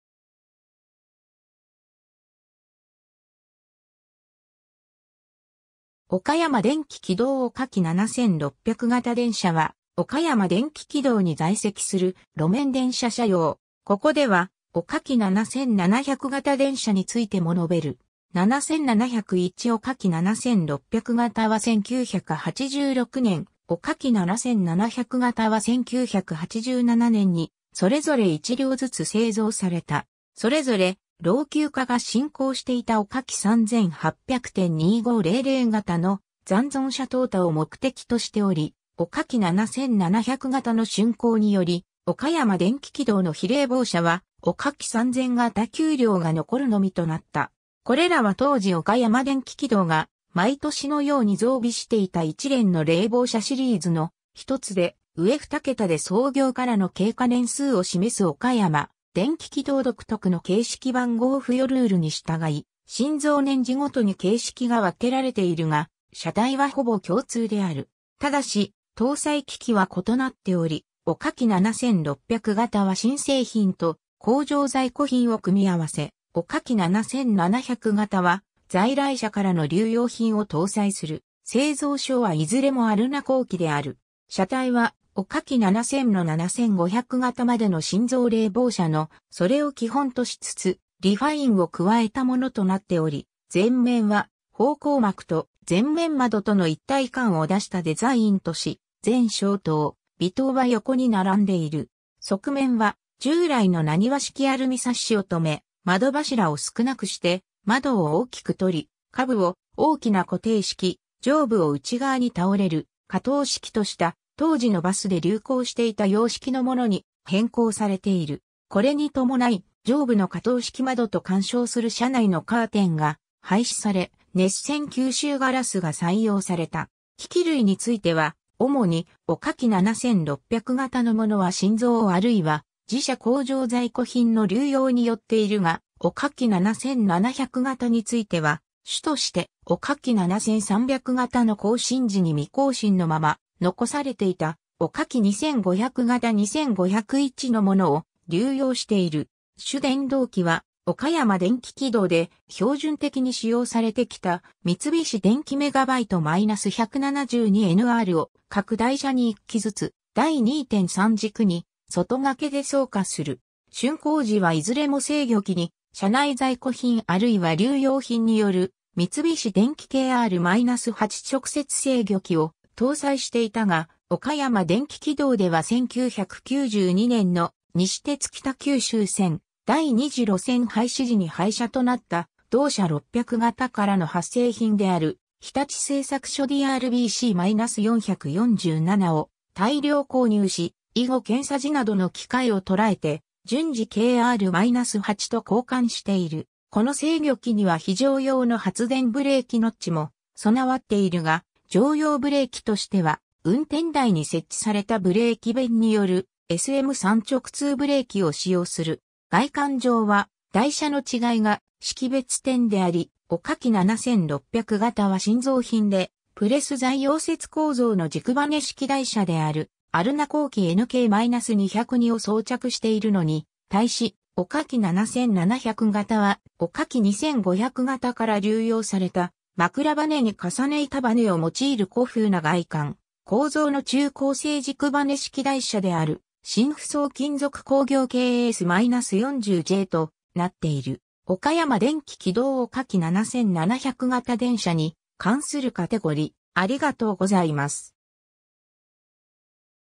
岡山電気軌道岡軌7600型電車
岡山電気軌道岡軌7600型電車（おかやまでんききどうおかき7600がたでんしゃ）は、岡山電気軌道に在籍する路面電車車両。
ここでは、岡軌7700型電車についても述べる。
 7701（岡軌7600・7700型は同一形態。外観上は台車のみ異なる） 岡軌7600型は1986年（昭和61年）、岡軌7700型は1987年（昭和62年）にそれぞれ1両ずつ製造された。
 それぞれ、老朽化が進行していた岡軌3800・2500型の残存車淘汰を目的としており、岡軌7700型の竣工により岡山電気軌道の非冷房車は岡軌3000型9両が残るのみとなった。
 これらは当時岡山電気軌道が毎年のように増備していた一連の冷房車シリーズの一つで、上2桁で創業からの経過年数を示す岡山電気軌道独特の形式番号付与ルールに従い、新造年次ごとに形式が分けられているが、車体はほぼ共通である。
 ただし、搭載機器は異なっており、岡軌7600型は新製品と工場在庫品を組み合わせ、岡軌7700型は在来車からの流用品を搭載する。
 製造所はいずれもアルナ工機である。
 車体は、岡軌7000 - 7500型までの新造冷房車のそれを基本としつつ、リファインを加えたものとなっており、前面は方向幕と前面窓との一体感を出したデザインとし、前照灯・尾灯は横に並んでいる。
側面は従来のナニワ式アルミサッシを止め、窓柱を少なくして窓を大きくとり、下部を大きな固定式、上部を内側に倒れる可倒式とした、当時のバスで流行していた様式のものに変更されている。
これにともない、上部の可倒式窓と干渉する車内のカーテンが廃止され、熱線吸収ガラスが採用された。
 機器類については、主に岡軌7600型のものは新造あるいは自社工場在庫品の流用によっているが、岡軌7700型については主として岡軌7300型の更新時に未更新のまま残されていた、岡軌2500型2501（元呉市電700形702）のものを流用している。
 主電動機は岡山電気軌道で標準的に使用されてきた三菱電機MB-172NRを各台車に1基ずつ、第2・3軸に外掛けで装架する。
 竣工時はいずれも制御器に社内在庫品あるいは流用品による三菱電機KR-8直接制御器を搭載していたが、岡山電気軌道では1992年の西鉄北九州線第2次路線廃止時に廃車となった同社600形からの発生品である日立製作所DR BC-447を大量購入し、以後検査時などの機会をとらえて順次KR-8と交換している。
 この制御器には非常用の発電ブレーキノッチも備わっているが、常用ブレーキとしては運転台に設置されたブレーキ弁によるSM3直通ブレーキを使用する。
 外観上は台車の違いが識別点であり、岡軌7600型は新造品でプレス材溶接構造の軸ばね式台車であるアルナ工機NK-202を装着しているのに対し、岡軌7700型は岡軌2500型から流用された、枕ばねに重ね板ばねを用いる古風な外観・構造の鋳鋼製軸ばね式台車である新扶桑金属工業KS-40J（H-2127）となっている。
 岡山電気軌道岡軌7700型電車に関するカテゴリ: